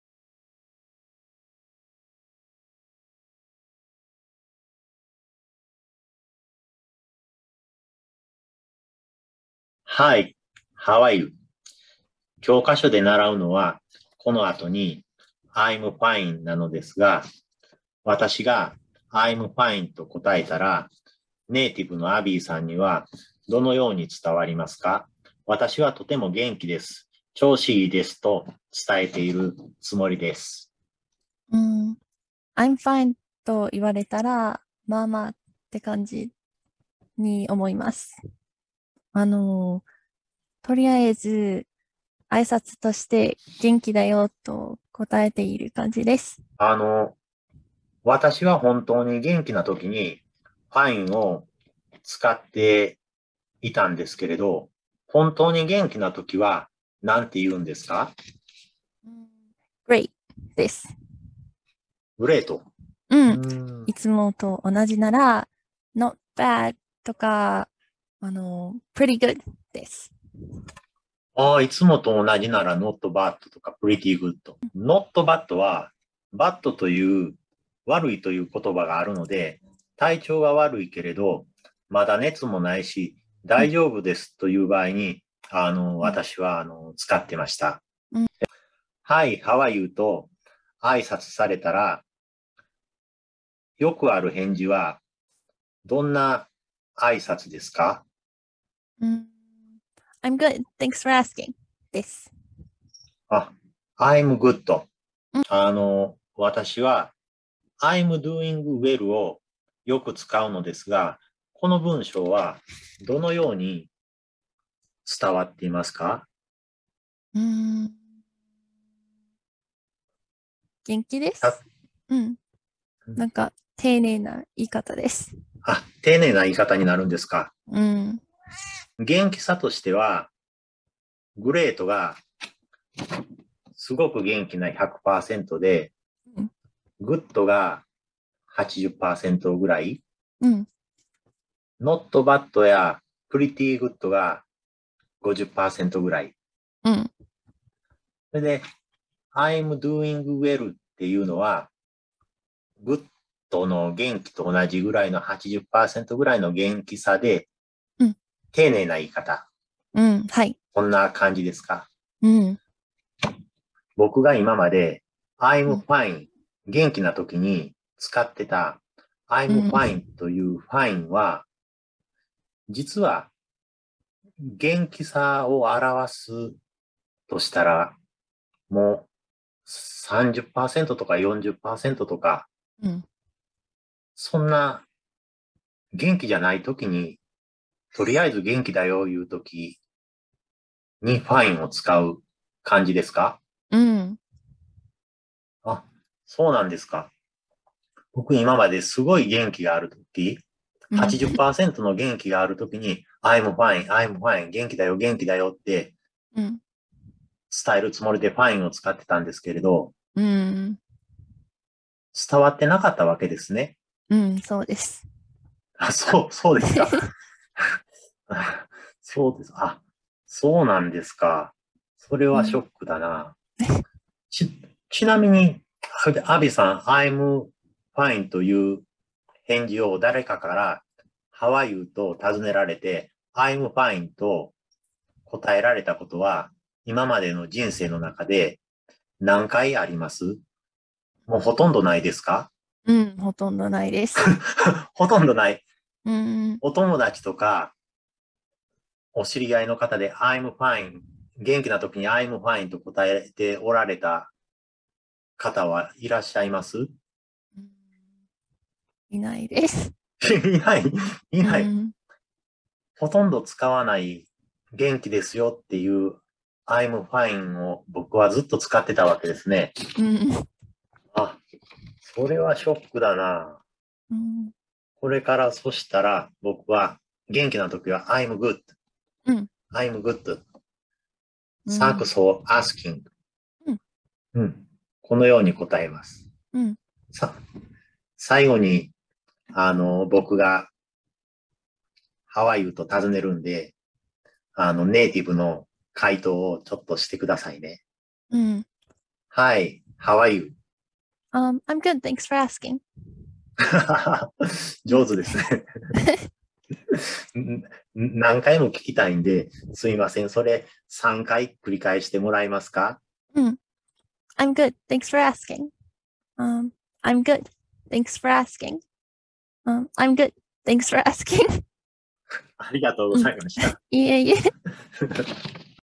【英語緊急速報】本当に元気なときには、I'm fineは使わない！－恥ずかしがり屋の英語教室
カリフォルニアにお住まいのNative Speakerで、とても恥ずかしがり屋のAbbyさんと53才の英語が出来ない恥ずかしがり屋のおじさんの英語教室。

今日は I'm fine　元気ですの意外なニュアンスをお伝えします。